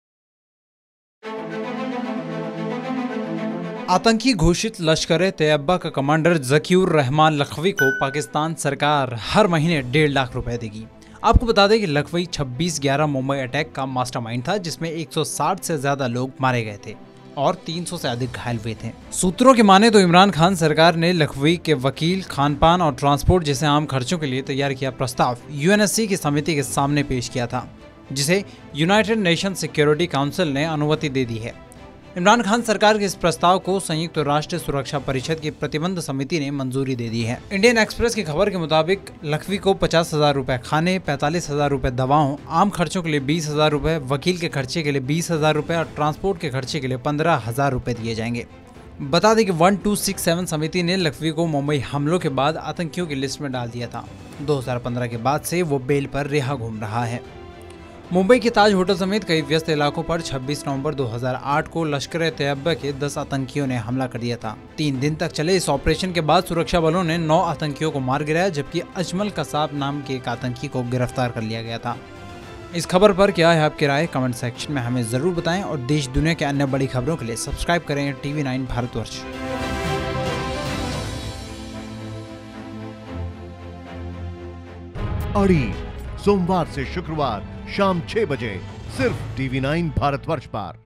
Lashkar आतंकी Lakhvi का खर्च उठाएगा Pakistan, UN की मंजूरी के बाद देगा 1.5 लाख रुपए
आतंकी का खर्च उठाएगा पाकिस्तान !

आतंकी घोषित लश्कर-ए-तैयबा का कमांडर जकीउर रहमान लखवी को पाकिस्तान सरकार हर महीने डेढ़ लाख रुपये देगी. आपको बता दें कि लखवी 26/11 मुंबई अटैक का मास्टर माइंड था जिसमें 160 से ज्यादा लोग मारे गए थे और 300 से अधिक घायल हुए थे.